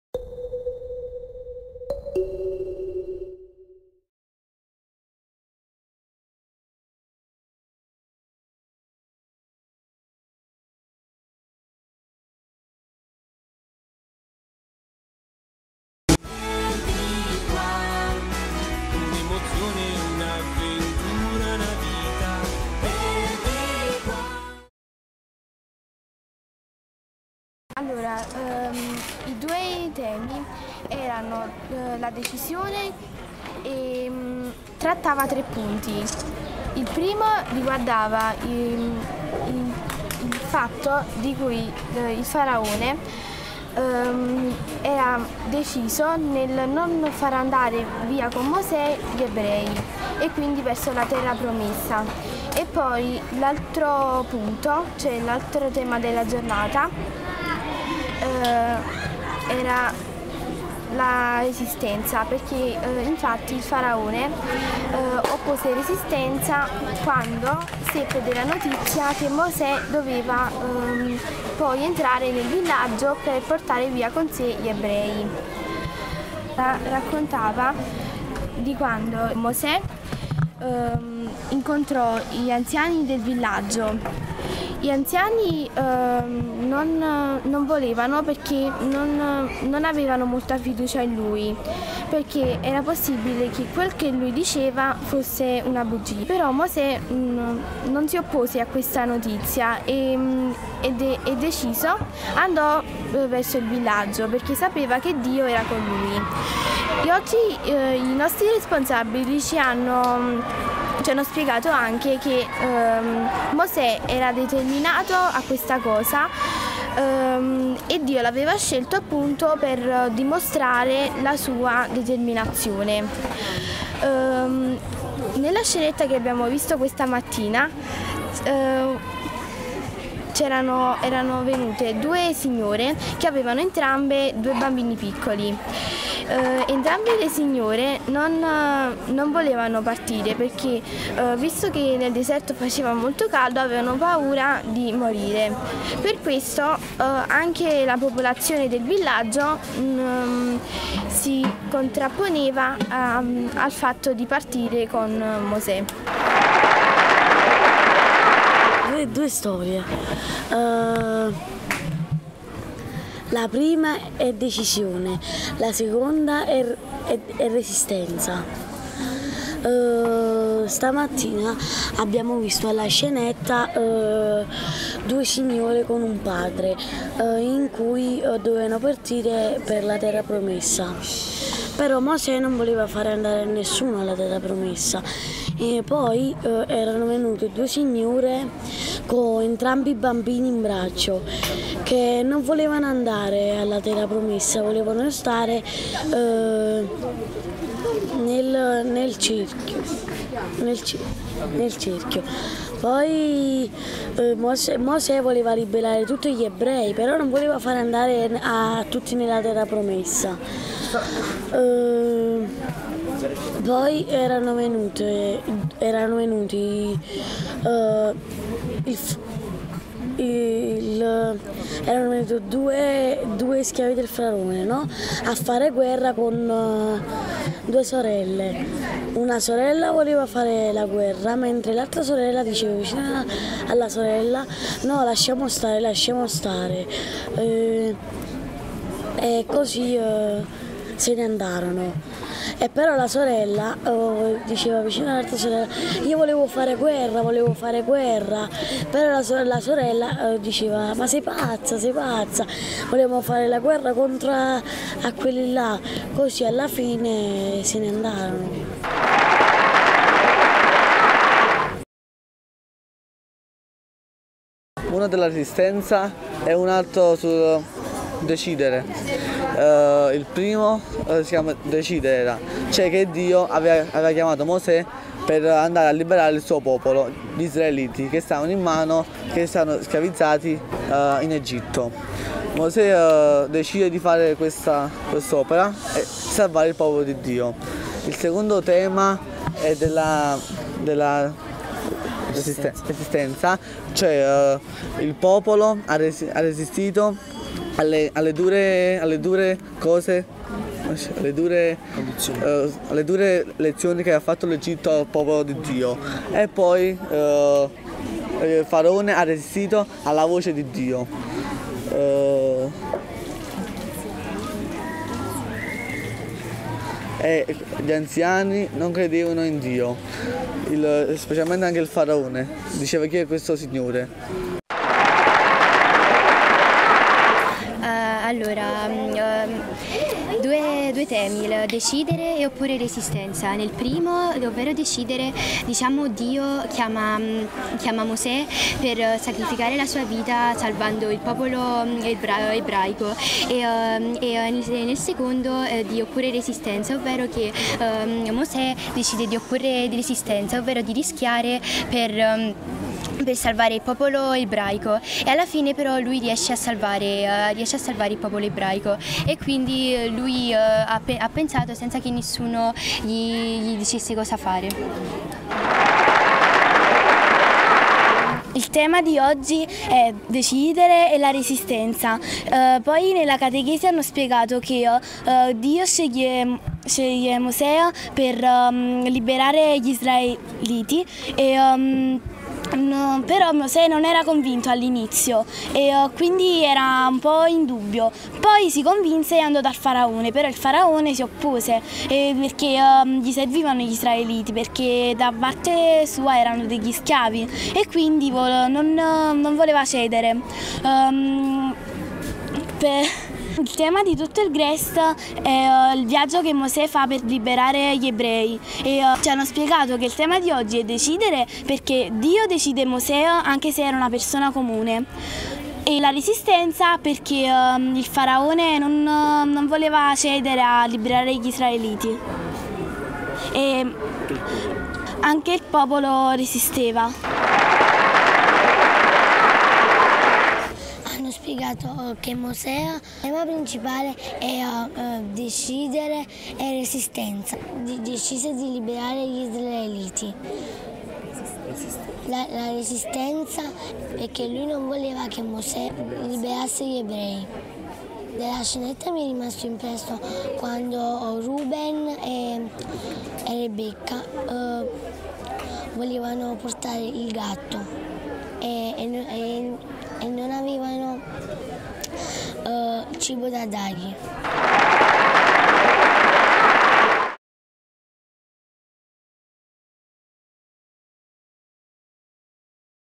DIARIO DI BORDO - GREST 2016 (PER DI QUA)
MINI CLIP SPERIMENTALE SUGLI ARGOMENTI TRATTATI DURANTE LA CATECHESI DELLA MATTINA DOVE IL PROTAGONISTA E' UN MEMBRO DI OGNI SQUADRA.
                                 MADONNA DI CASANDRINO